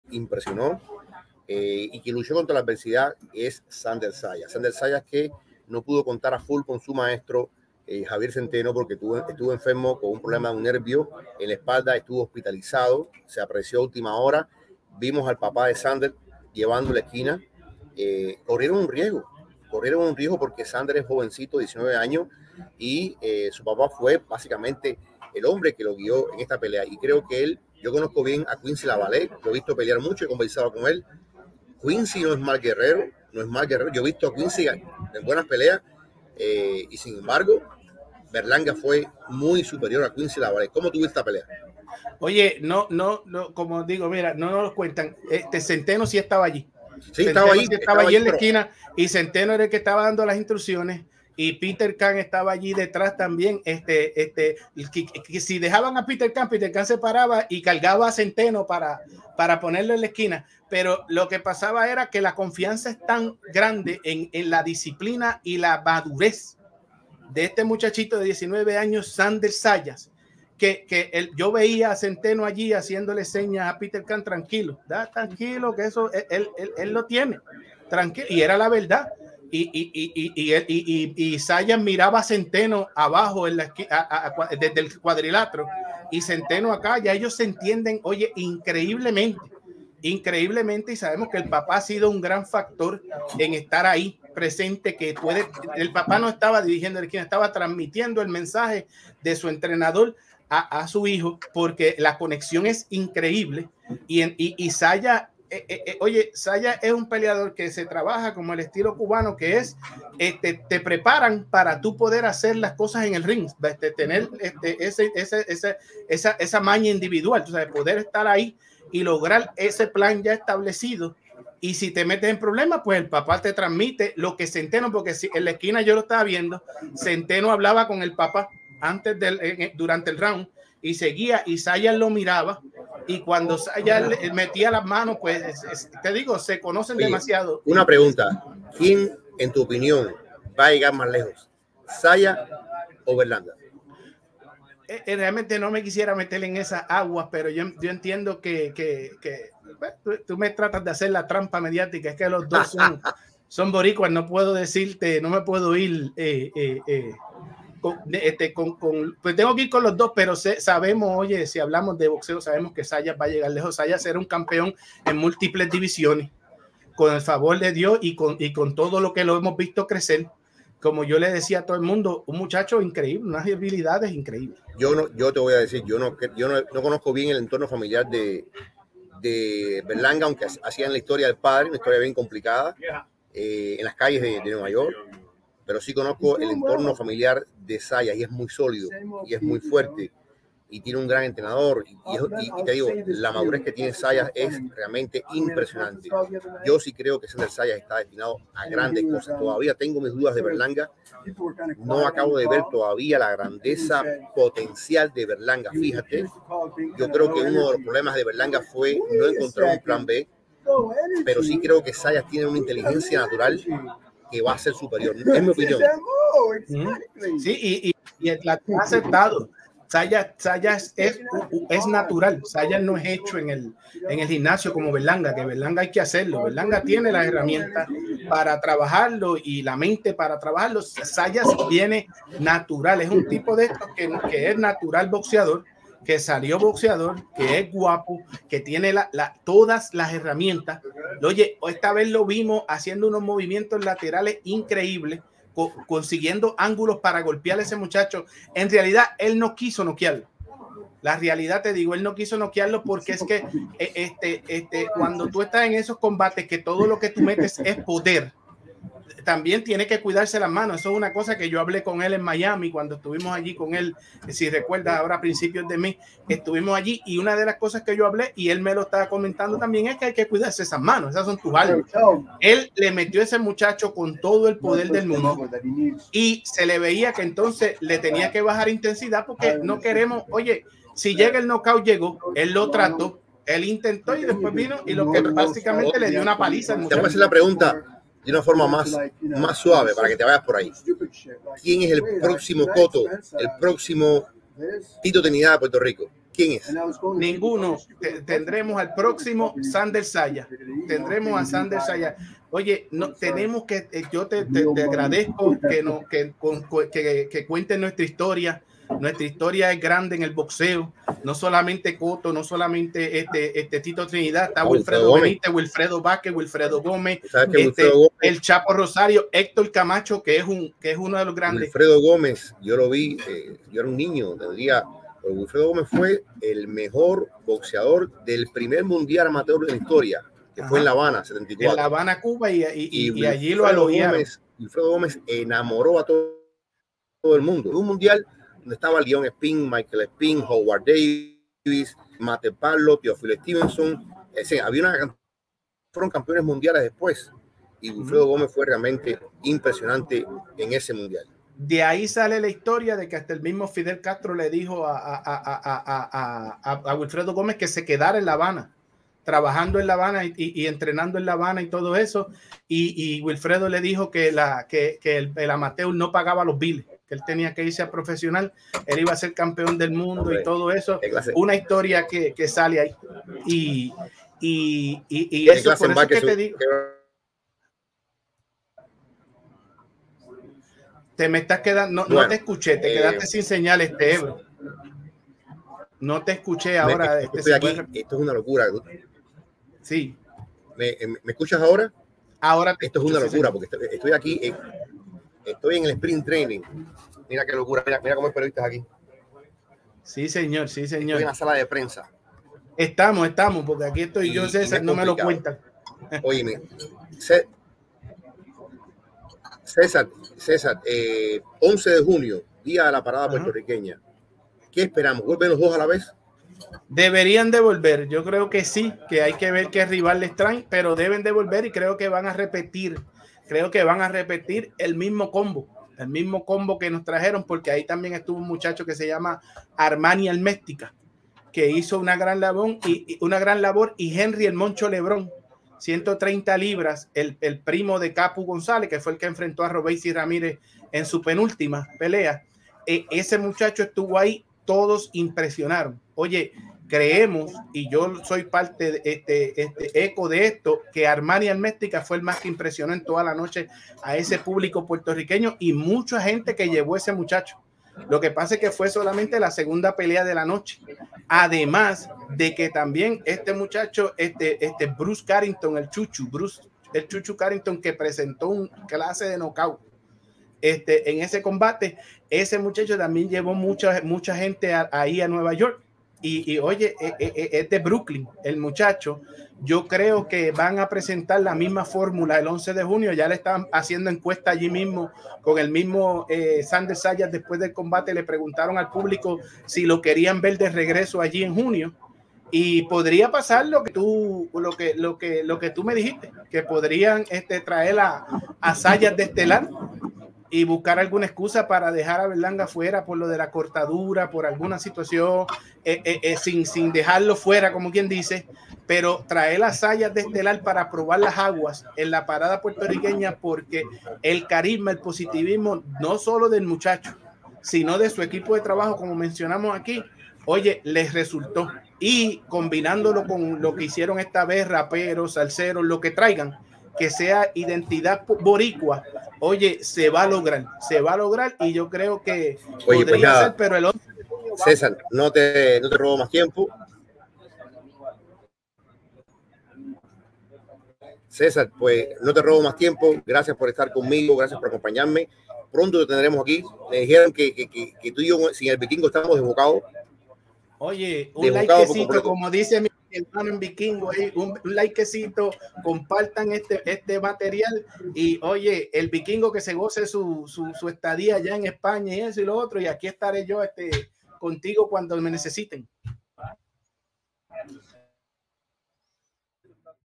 ¿ZAYAS ES MÁS PROSPECTO QUE BERLANGA QUE NO DESPEGÓ EN NUEVA YORK?
Ahora mismo hay un debate en el mundo del boxeo sobre si Edgar Berlanga es mejor prospecto o no que su paisano boricua Xander Zayas, tras salir triunfadores ambos el pasado sábado en la ciudad de Nueva York.
Zayas parece seguir subiendo, aunque se espera mucho de su figura y por eso a veces nos quedamos con las ganas de ver más.
Con Berlanga, el debate es diferente porque ahora mismo da la impresión de que se ha estancado y que tras 16 nocauts de manera consecutiva en el primer asalto para comenzar su carrera, no es el mismo que en las últimas 3 peleas ha ganado por decisión.
El debate lo tenemos en El Vikingo y Ebro Live, con Jorge Ebro, de Serebro en los Deportes, y César Seda, quien estuvo en Nueva York para este gran evento de Top Rank que emocionó a miles de puertorriqueños y latinos presentes.
No pueden perderse este análisis porque hay sentimientos encontrados y no nos ponemos de acuerdo sobre este tema.